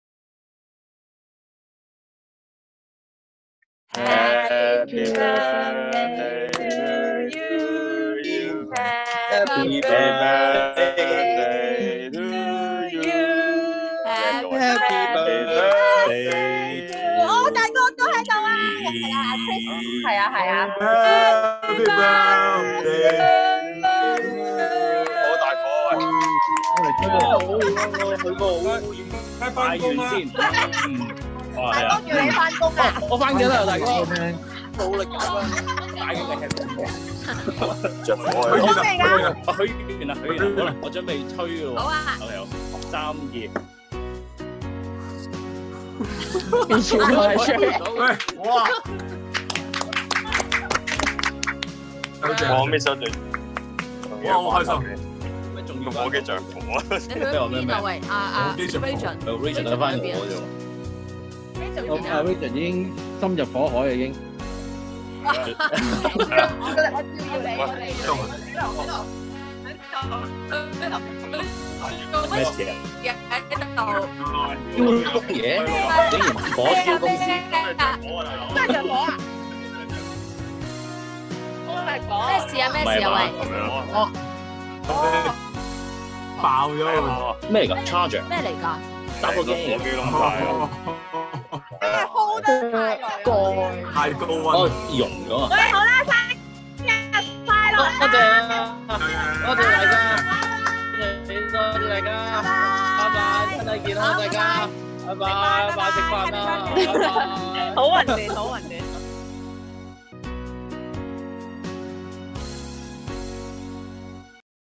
【Chris's Birthday ZOOM Party】之Reijen 好惹火🔥
電台同事們隔著畫面同CHRIS慶祝生日
Happy Birthday Chris!!
抱歉 畫面有一點卡